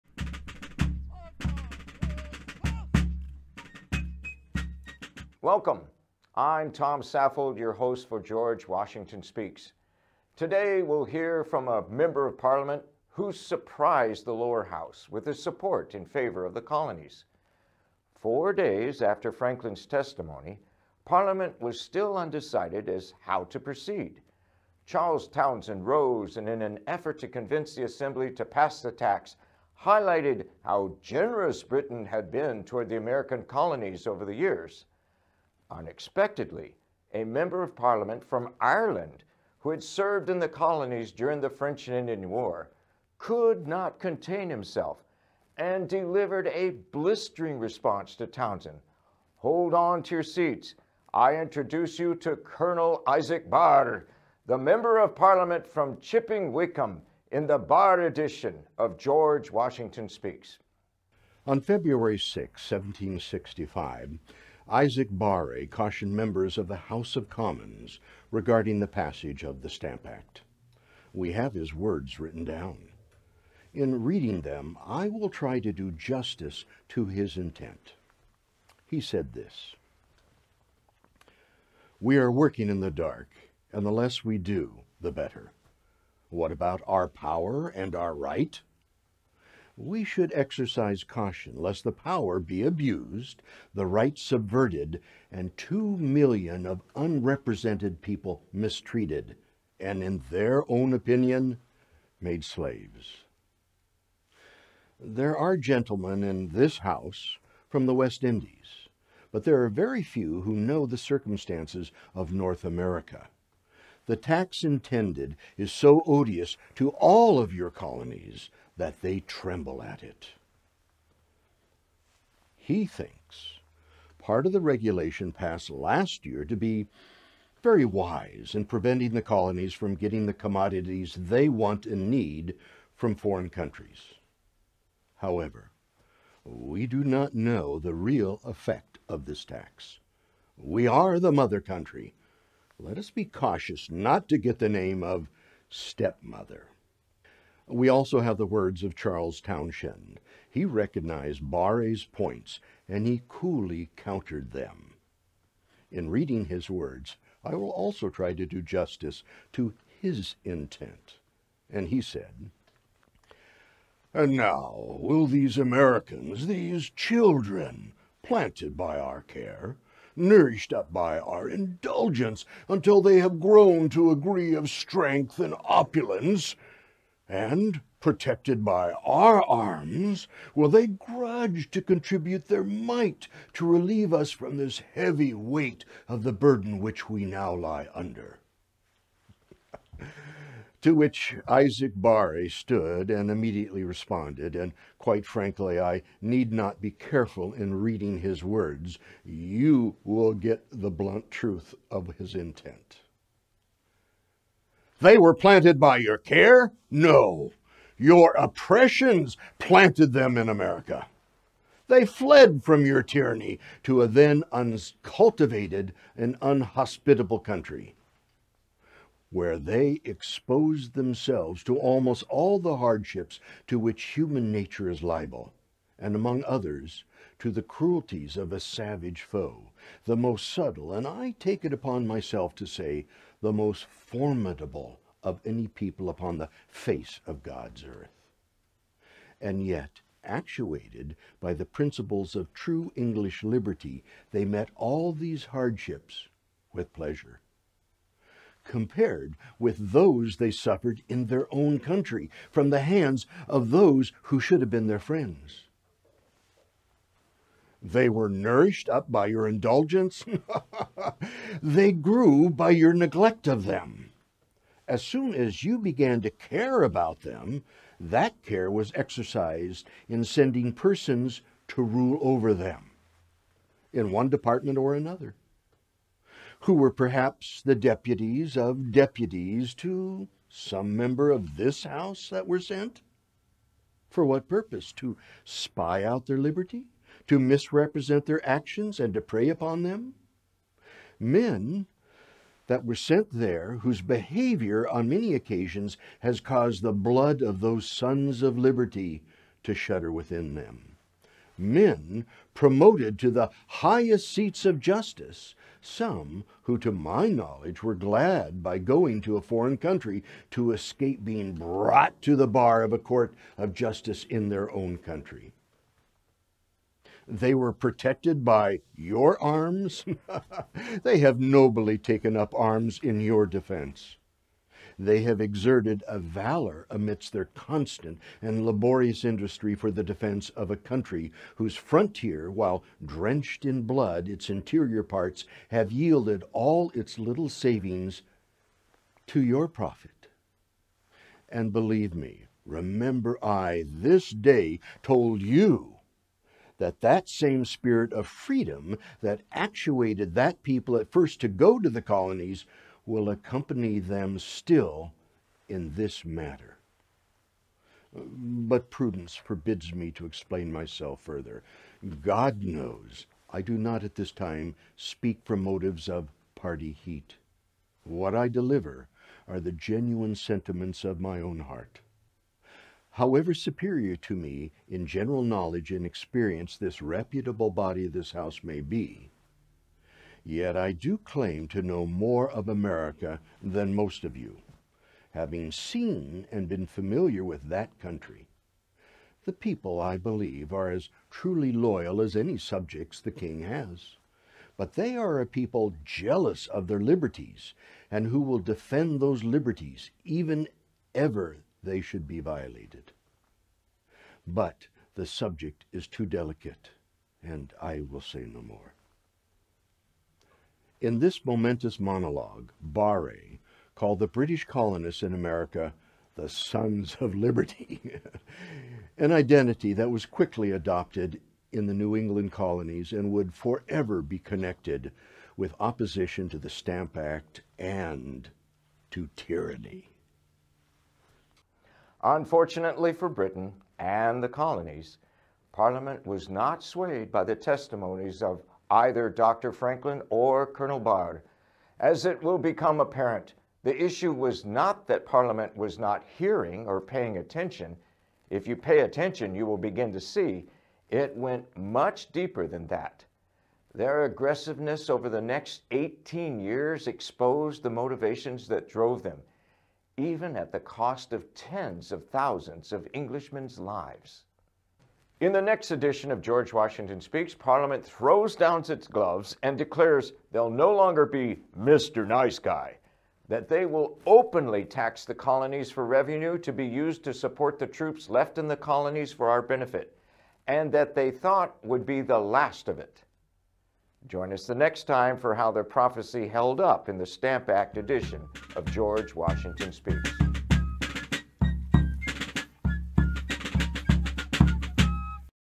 Isaac Baare Stands Up For The Colonies - February 6, 1765
George Washington Speaks describes how Isaac Baare Stood Up For The Colonies - February 6, 1765